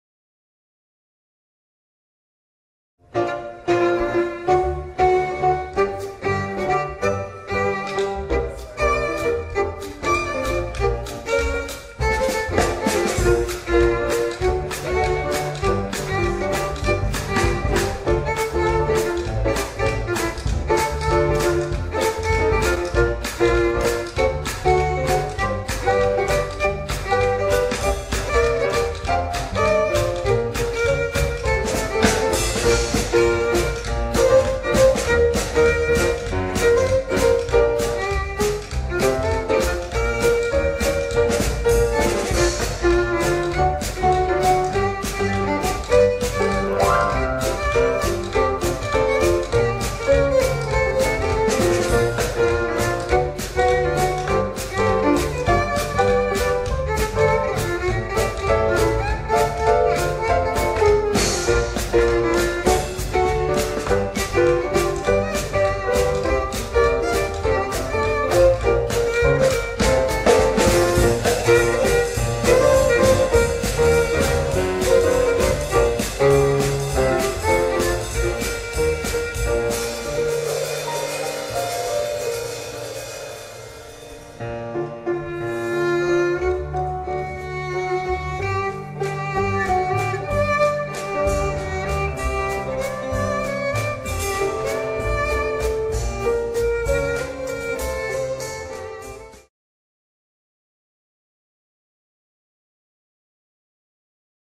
Charlestón. "Slapstick a ritmo de jazz" con The Silent Entertainers Band
"Slapstick a ritmo de jazz" Cine y jazz para toda la familia en el formato de cine concierto. Con repertorios originales rescatados del acompañamiento musical del cine "mudo" y el primero ragtime y jazz de los felices años 20